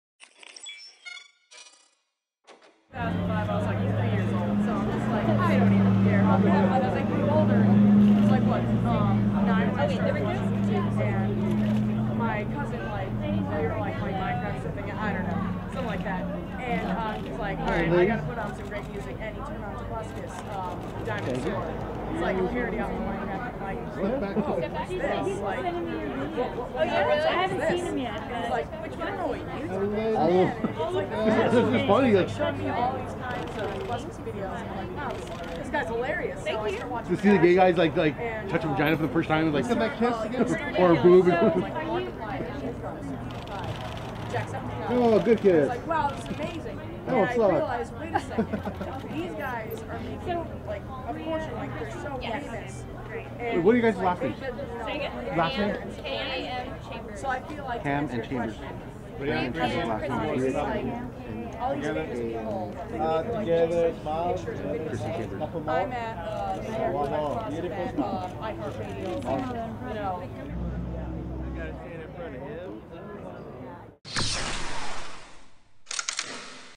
BriaAndChrissy "Tubeathon 2016" Red Carpet
BROLL: Lesbian duo BriaAndChrissy arrives on the red carpet at What’s Trending 4th annual Tubeathon digital telethon event benefiting the American Red Cross held at iHeartRadio Theater in Burbank, Ca April 20, 2016